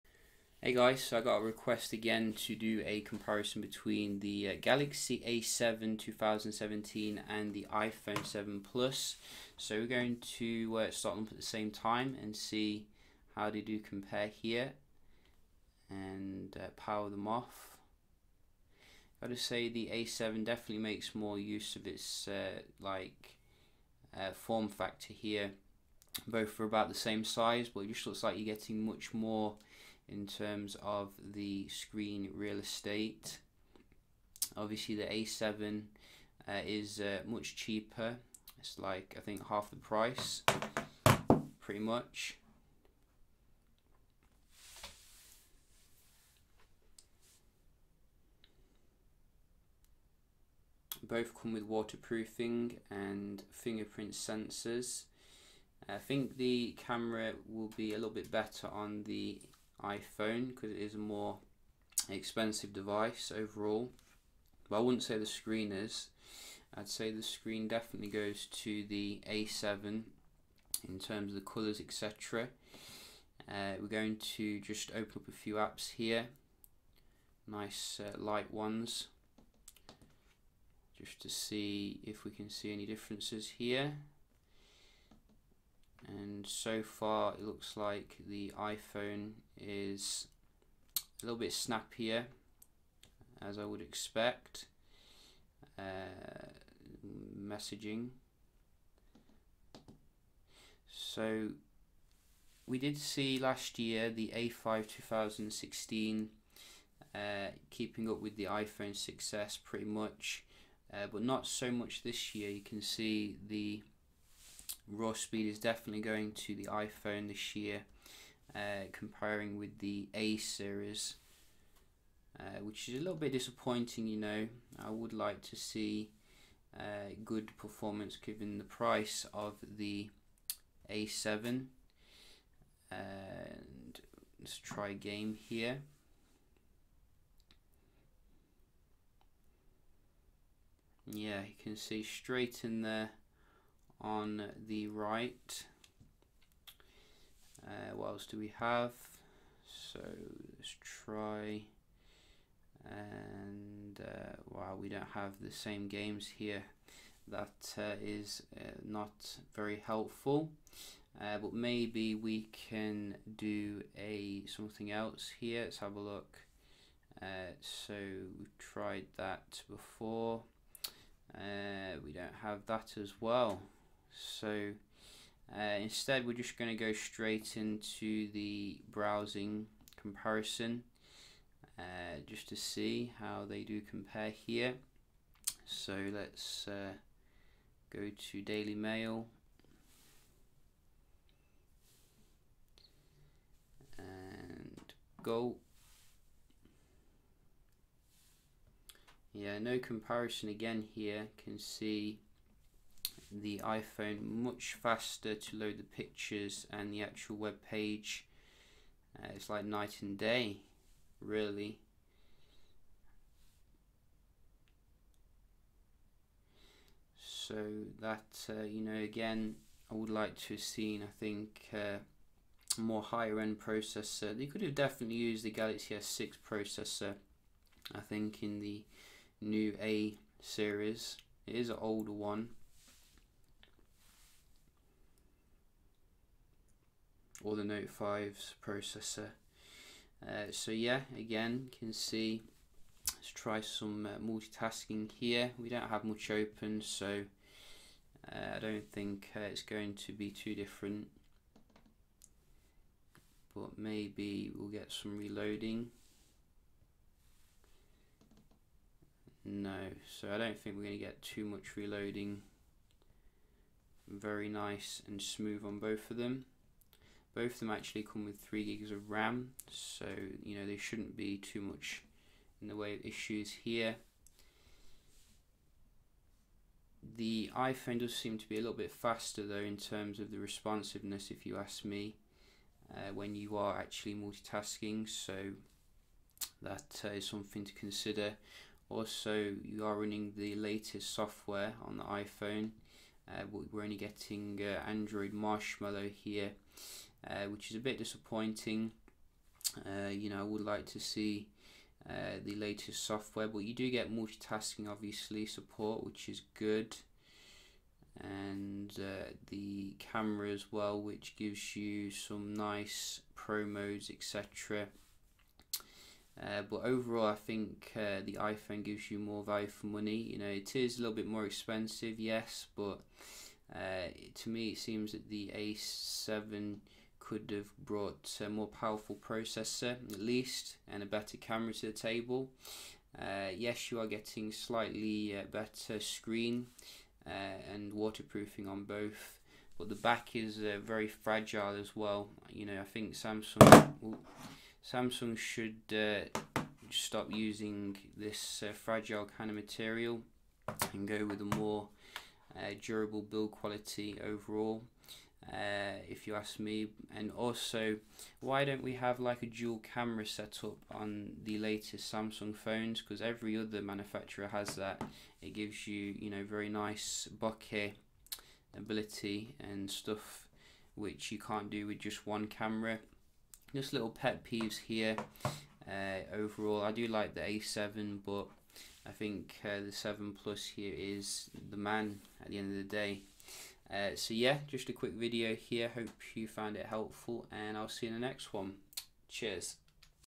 Samsung Galaxy A7 2017 vs iPhone 7 Plus - Which is Fastest?
Speed test of Samsung Galaxy A7 2017 vs iPhone 7 Plus - Which is Fastest?